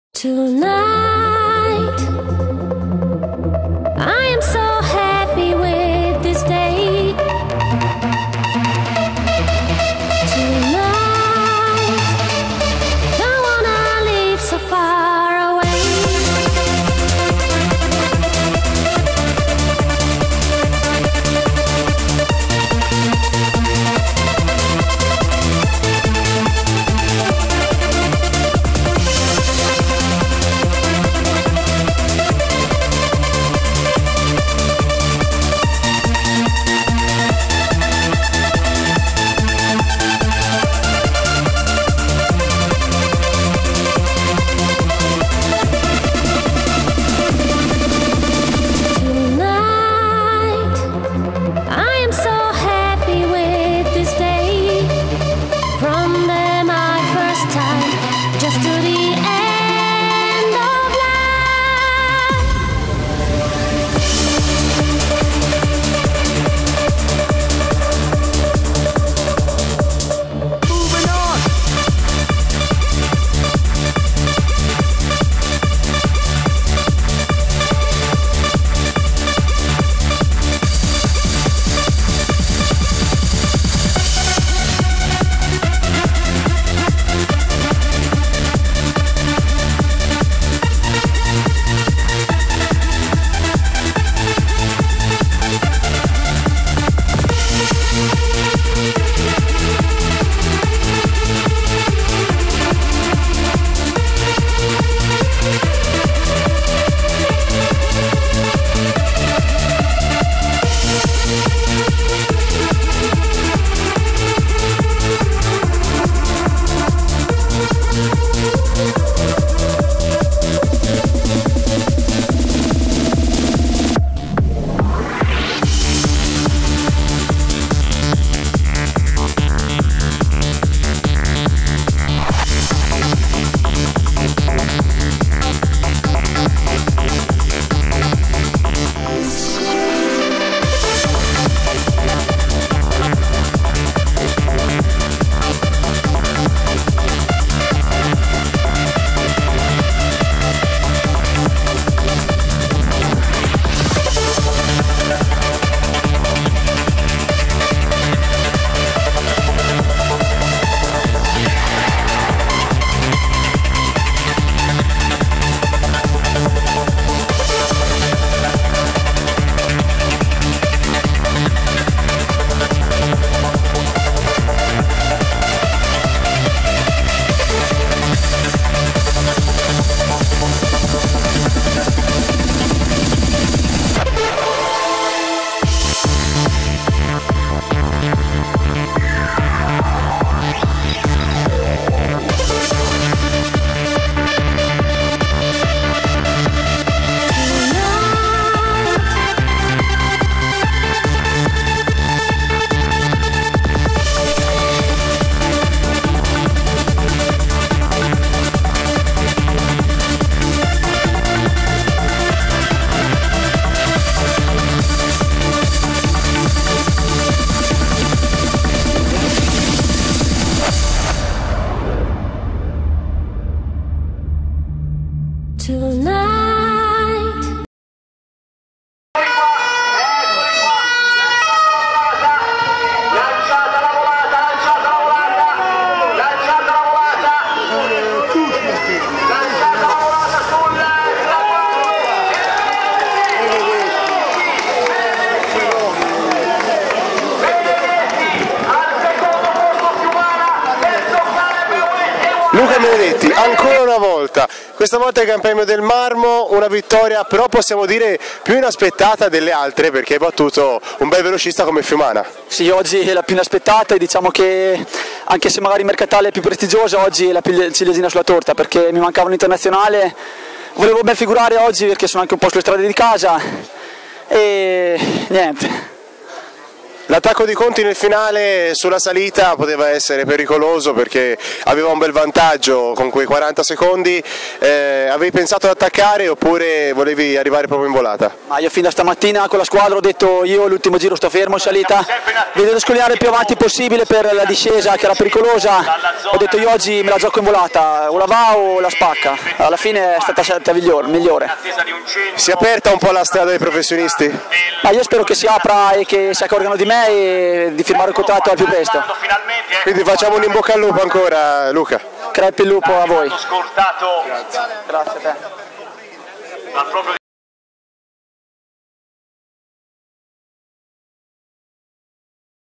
Luca Benedetti Senza Limiti Al GP Del Marmo
Sesto centro stagionale per Luca Benedetti che batte il vincitore dell'anno prima Thomas Fiumana. Un risultato straordinario che forse aprirà le porte del professionismo